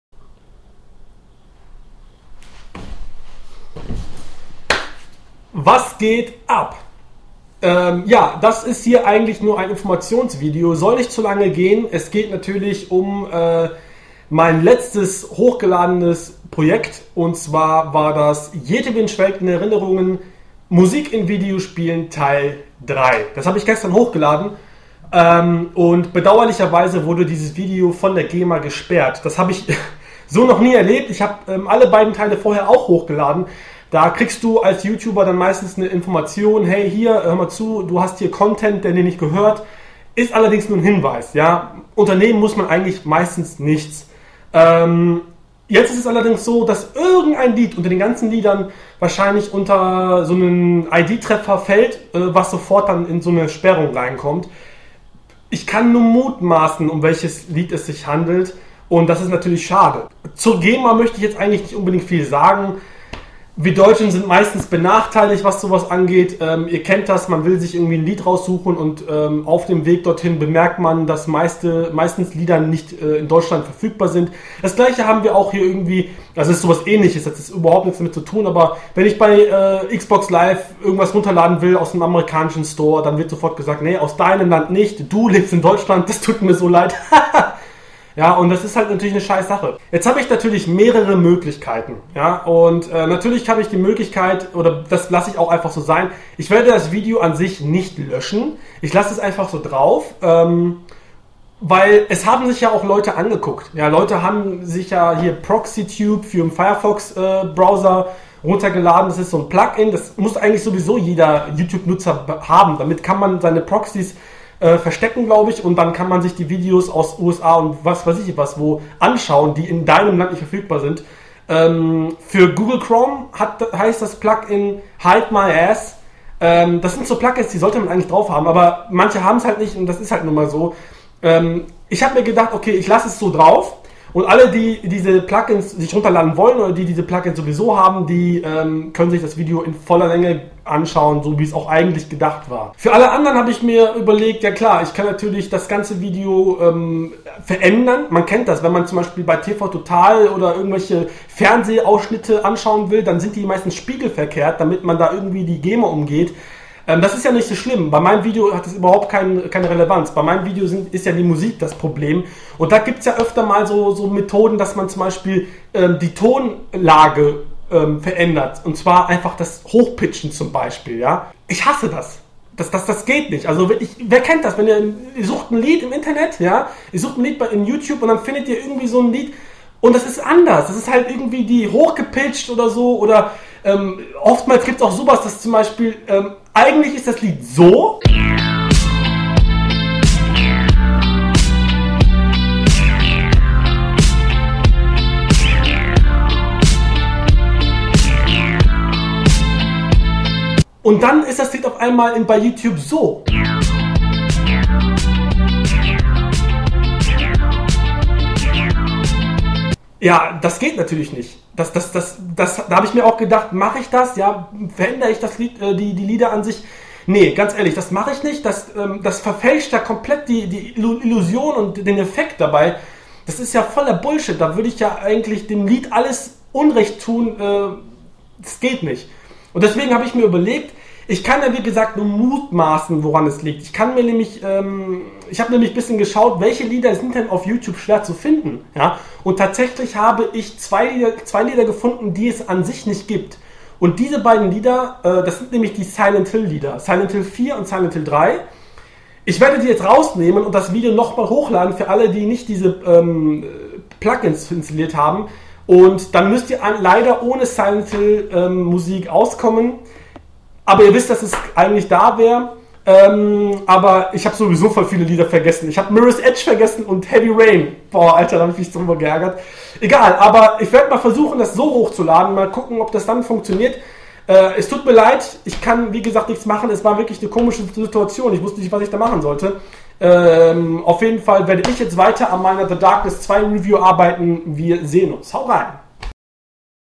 GEMA: Lasst mich doch in Ruhe!
Mein Problem mit meinem letzten Video wird euch sicherlich bekannt sein,oder?
Meine Meinung dazu und was ich unternehmen kann.

Wenn ihr das Original Video schauen wollt, dann braucht ihr folgende Plugins:

Firefox: Proxytube
Chrome: HideMyAss